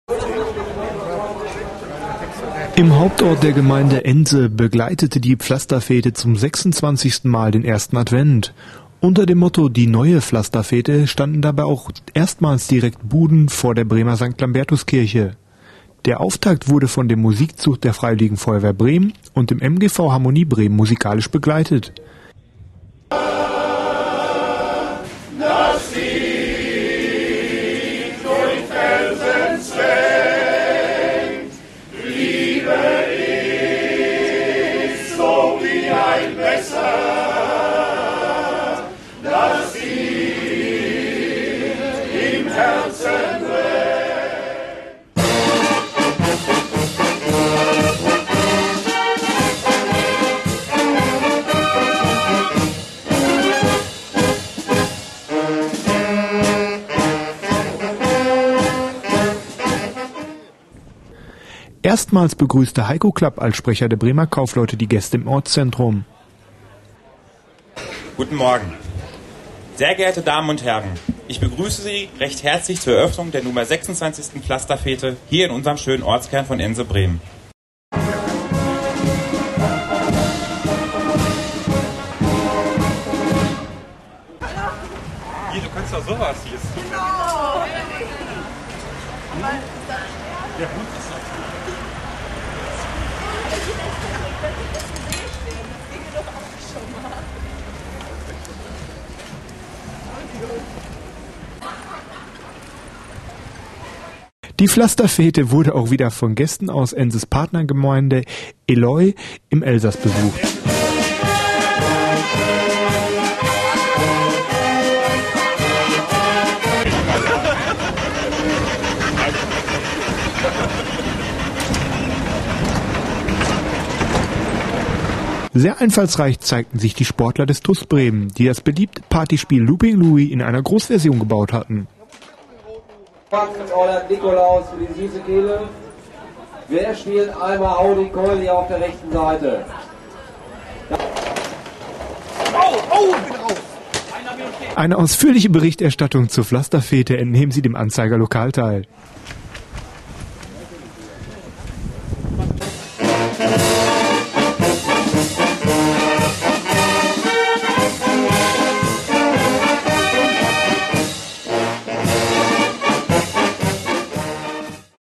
Pflasterfete Ense-Bremen 2009
Die Pflasterfete war lange Jahre eine Institution in Ense. Hier ein aus Video aus Ense-Bremen vom 1. Advent 2009.